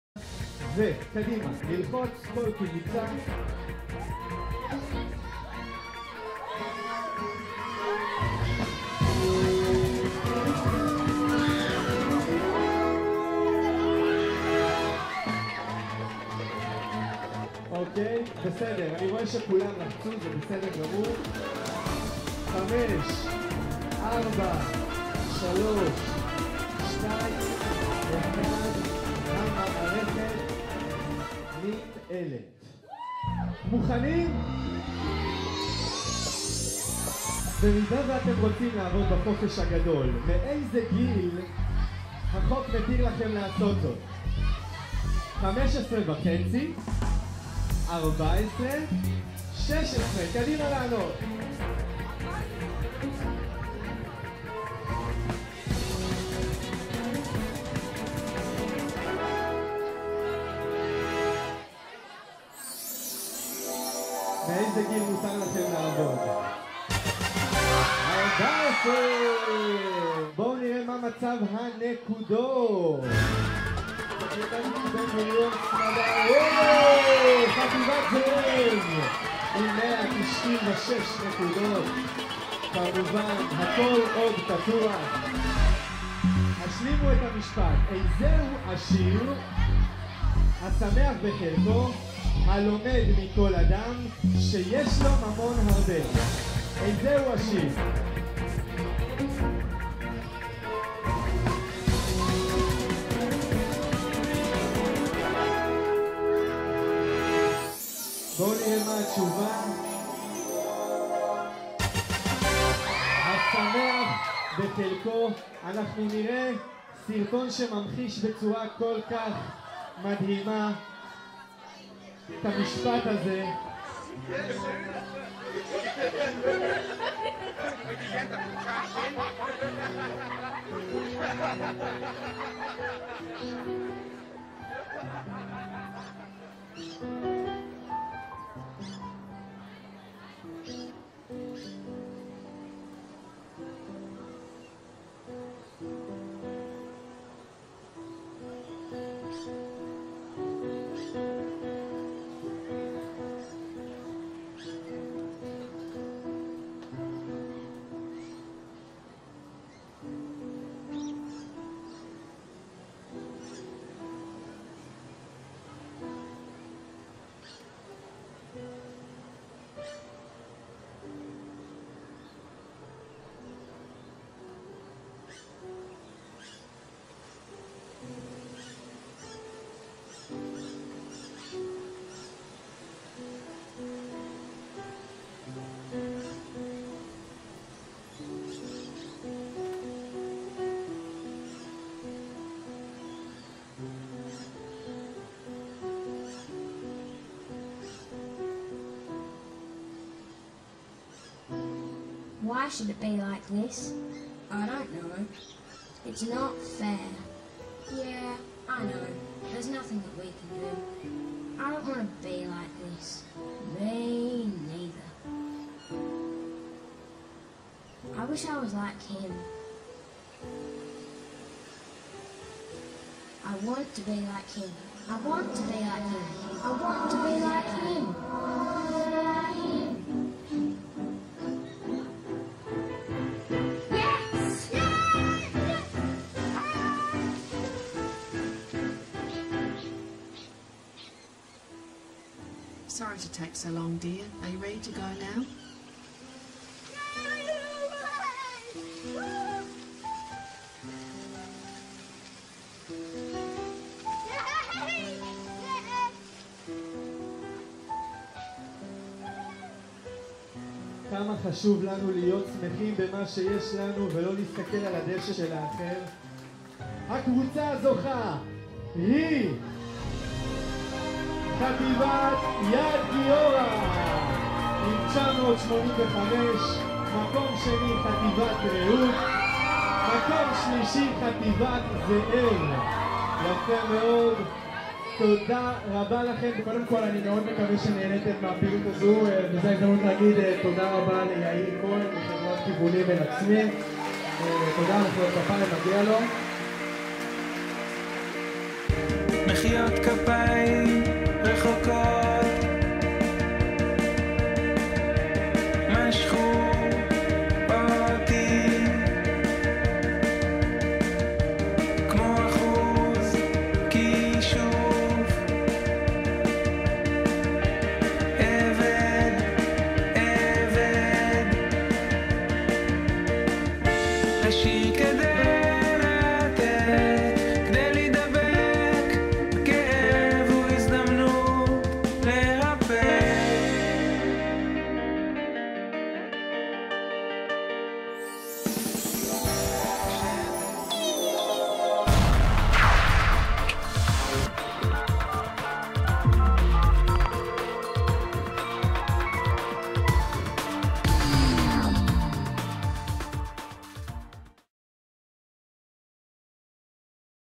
כיוונים אל עצמי - מועצות תלמידים הרצליה
מועצות תלמידים מ6 חטיבות בהרצליה הגיעו לסמנריון בקיסריה ונהנו מחידון טריוויה באמצעות שלטים
יאיר כהן הפקות מוסיקליות - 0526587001